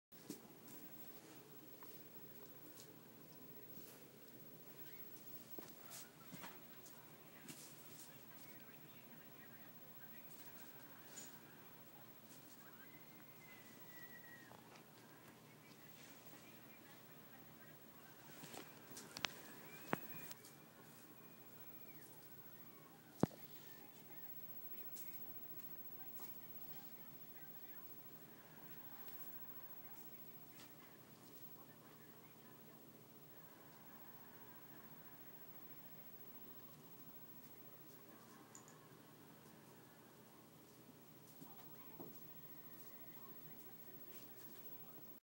Little kitten is super playful!
This is a Throwback video from when Jonah was only 2 months old! But He is still just as playful now!

Want more Jonah?
Vine- @Julia A and Jonah the kitten
Petcube- @JonahTheKitten